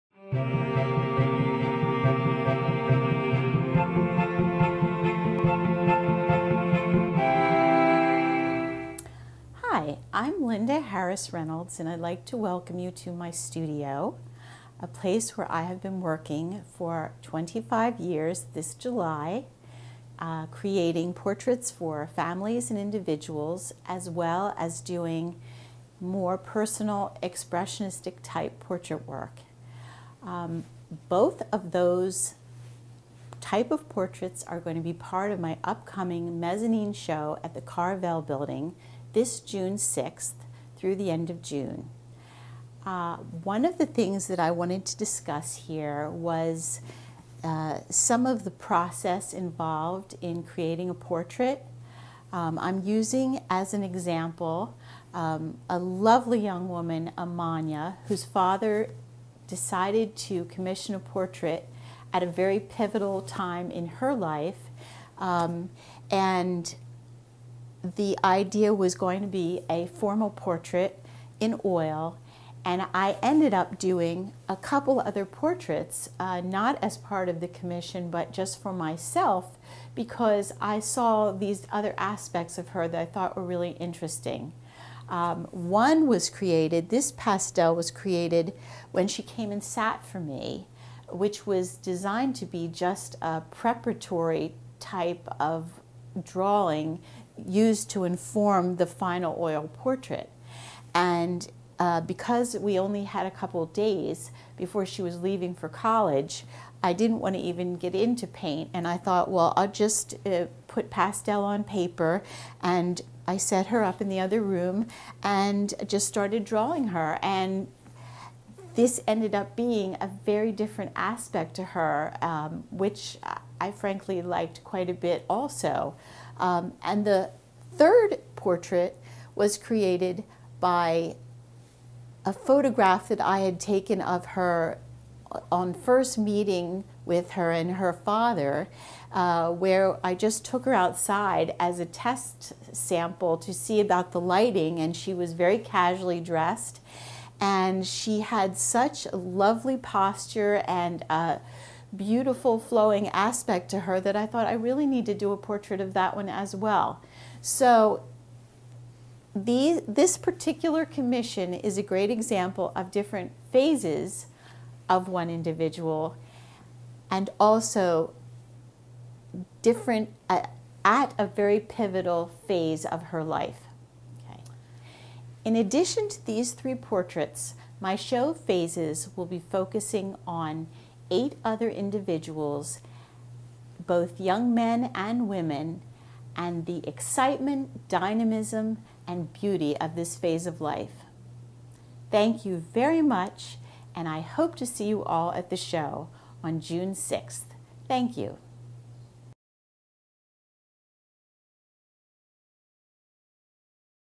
Meet the Artist - Linda Harris Reynolds
Introduction to Delaware artist Linda Harris Reynolds. Selected pieces of her work are on display during June, 2014, at the Mezzanine Gallery in Wilmington, Delaware.